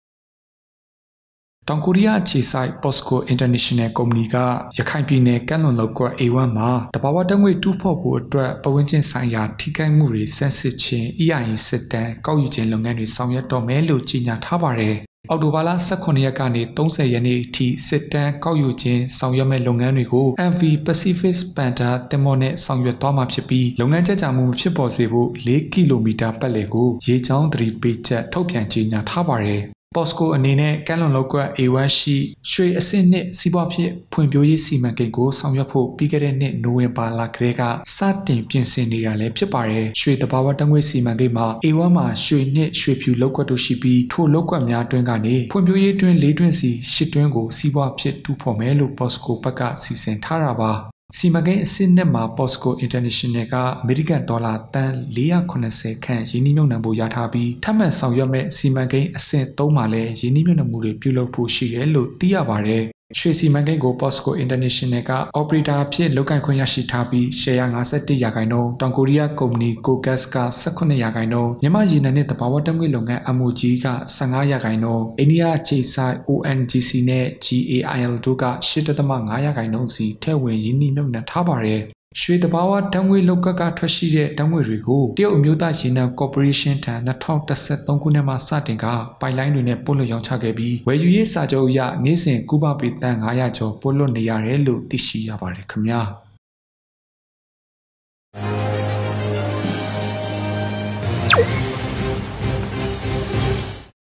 POSCO က ရခိုင်ကမ်းလွန်မှာရှိတဲ့ လုပ်ကွက် A-1 မှာ EIA Survey လုပ်ငန်း ဆောင်ရွက်ဖို့ ပြင်ဆင်နေ
ကိုရီးယား POSCO က ရခိုင်ကမ်းလွန်မှာရှိတဲ့ လုပ်ကွက် A-1 မှာ EIA Survey လုပ်ငန်း ဆောင်ရွက်ဖို့ ပြင်ဆင်နေပါပြီ

လုပ်ကွက် A-1 က ရွှေဓာတ်ငွေ့တွင်းစီမံကိန်းရဲ့ အစိတ်အပိုင်းတစ်ခုဖြစ်ပြီး ရွှေနှင့် ရွှေဖြူလုပ်ကွက်တို့ ပါဝင်နေပါတယ်။
ထိုလုပ်ကွက်တွေမှာ ဖွံ့ဖြိုးရေးတွင်း (၈) တွင်းကို စီးပွားဖြစ်တူးဖော်ဖို့ Posco International က စီစဉ်ထားပါတယ်။

ရွှေစီမံကိန်းမှာ အော်ပရေတာဖြစ်တဲ့ Posco က ၅၁ ရာခိုင်နှုန်း၊ KOGAS က ၁၇ ရာခိုင်နှုန်း၊ MOGE က ၁၅ ရာခိုင်နှုန်း၊ အိန္ဒိယအခြေစိုက် ONGC နဲ့ GAIL တို့က ၈.၅ ရာခိုင်နှုန်းစီ ရင်းနှီးမြှုပ်နှံထားပါတယ်။
သတင်း - BETV Business